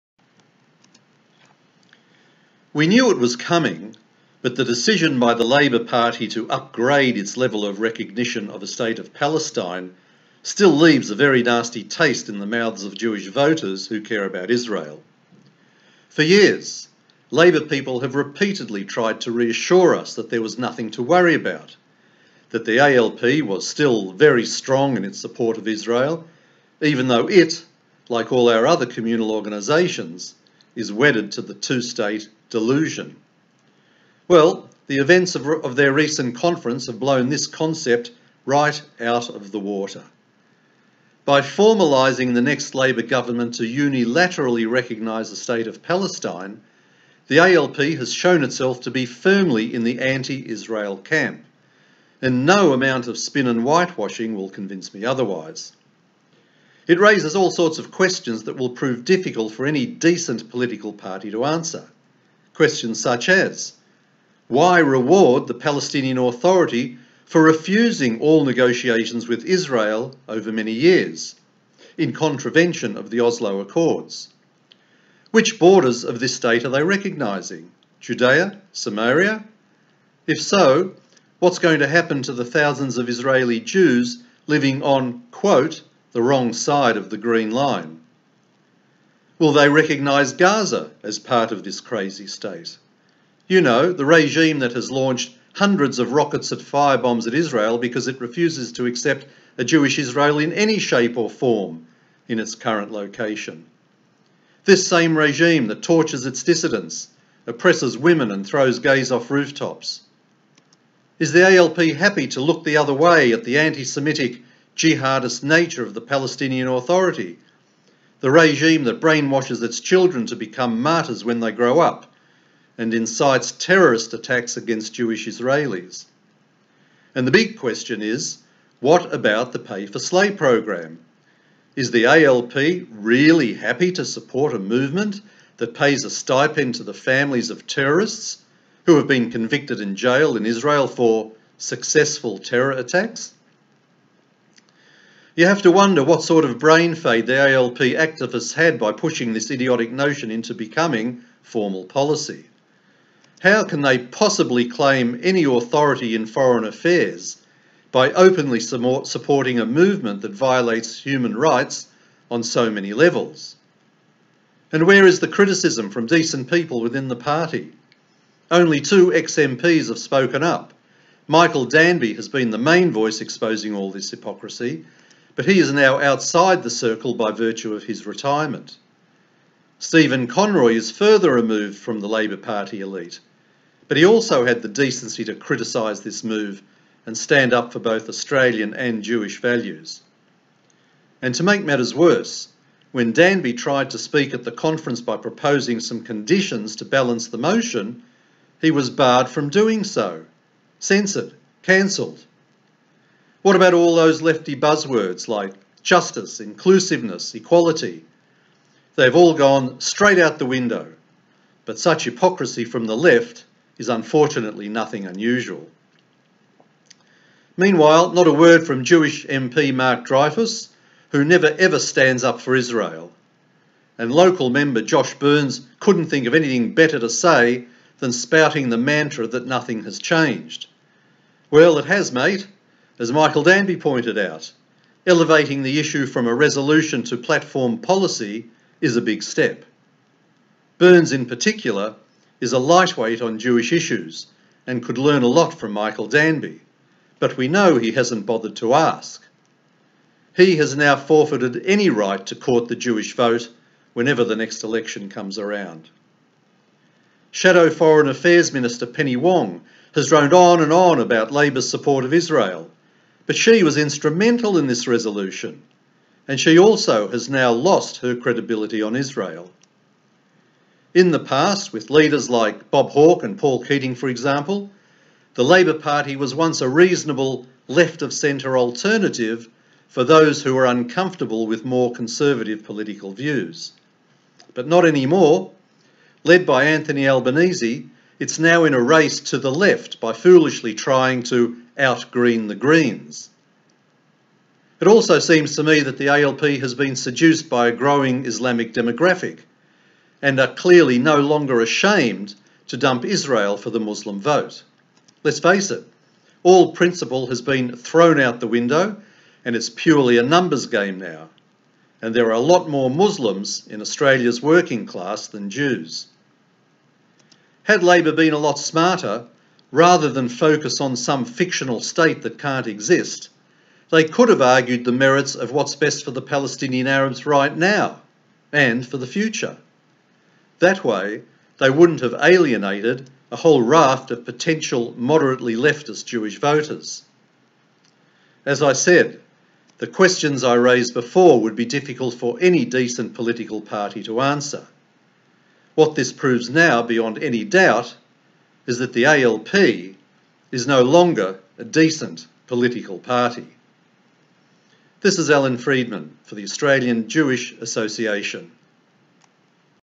ALP and recognition of Palestine Alan Freedman editorial
Another terrific editorial from AJA Vice-President Alan Freedman.
Just one of his conclusions from the decision taken at the Labor Party conference for unconditional recognition of a "State of Palestine";
After noting Labor has pushed aside many issues including the Hamas & PA involvement in terror, Alan states:
"All principle has been thrown out the window, it's now a numbers game and there are a lot more Muslims than Jews"